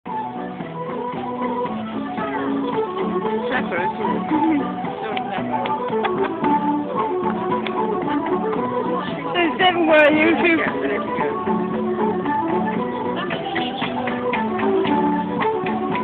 Dublin city dancing.
me and granny saw this silly man dancing